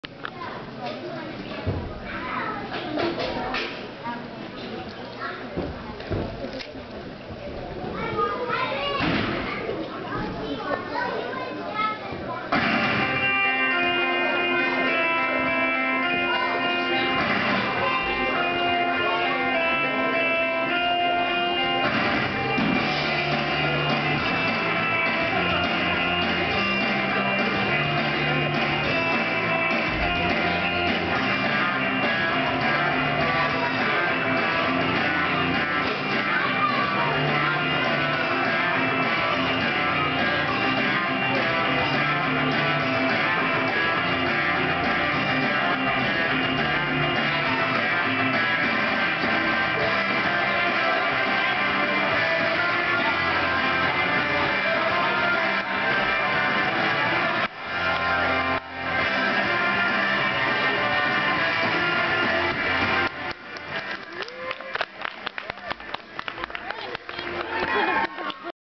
Morgyn's Gater Classic Beam routine
She did great!  Got a 7.3.  It was the first real meet, and beam was up first for them.  The last bit is me dropping the camera to clap like a crazy proud mamma.  lol.  No clue how to edit it and cut it out, and sot sure I want to anyway.  :)